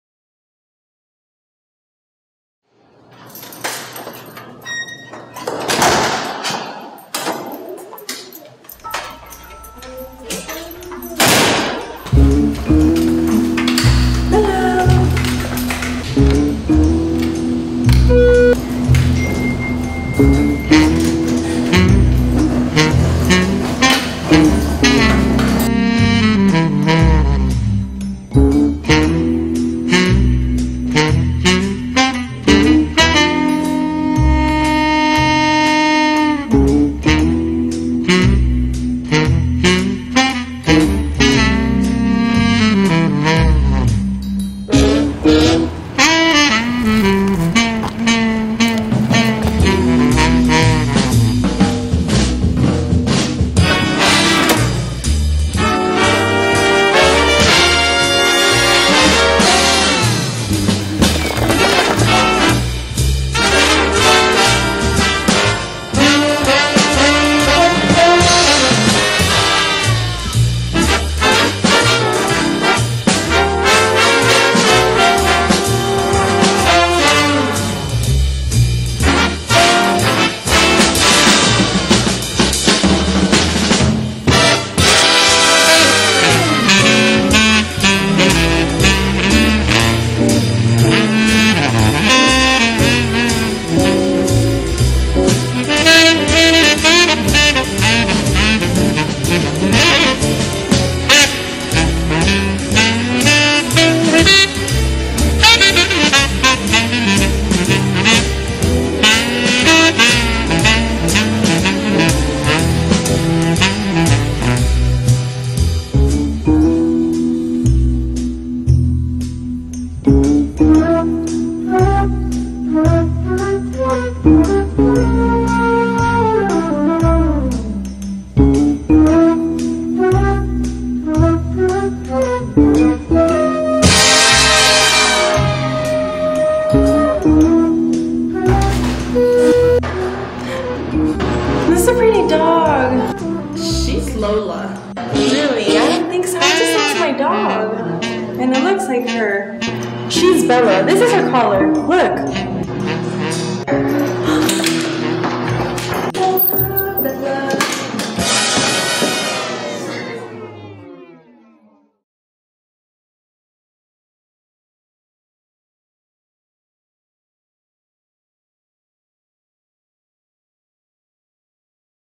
Taken by Valeria Reyes
a short film about the kidnapping of a dog.